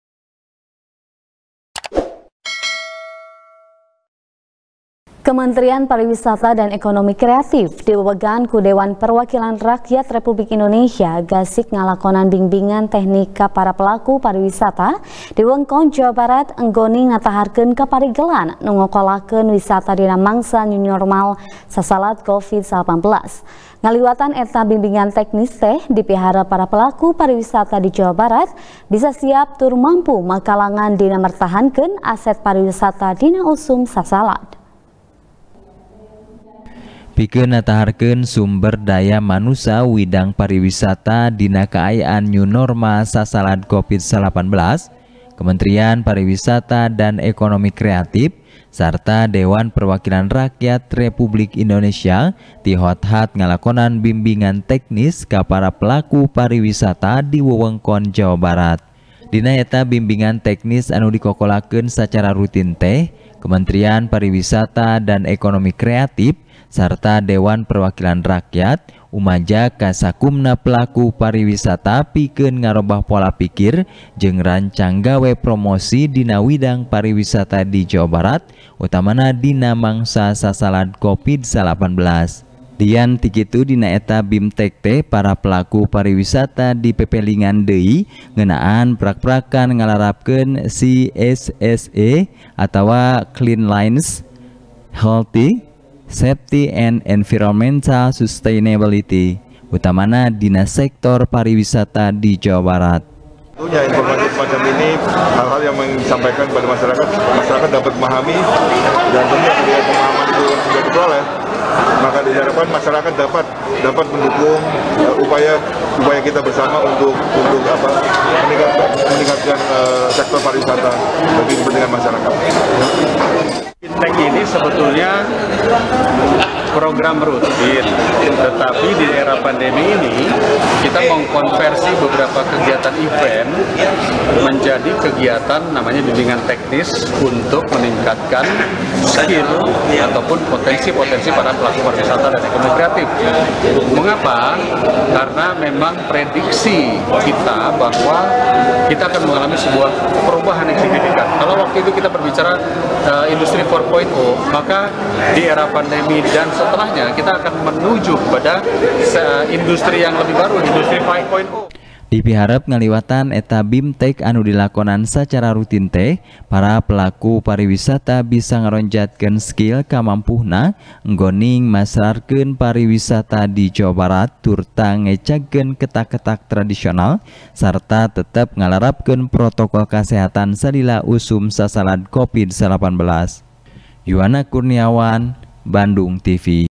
Ngalarapkeun CHSE Keur Pelaku Wisata Dina Mangsa Sasalad | TP | BANDUNGTV
TP  | BANDUNGTV | ASTRI CAHYANI | Kementrian Pariwisata Dan Ekonomi Kreatif, Diwewegan Ku Dewan Perwakilan Rakyat Republik Indonesia, Gasik Ngalakonan Bimbingan Teknis Ka Para Pelaku Pariwisata, Di Wewengkon Jawa Barat Enggoning Nataharkeun Kaparigelan Nu Ngarokolakeun Wisata Dina Mangsa New Normal Sasakad Covid19. Ngaliwatan Eta Bimbingan Teknis Teh, Dipiharep Para Pelaku Pariwisata Di Jawa Barat, Bisa Siap Tur Mampu Makalangan Dina Mertahankeun Aset Pariwisata, Dina Usum Sasalad.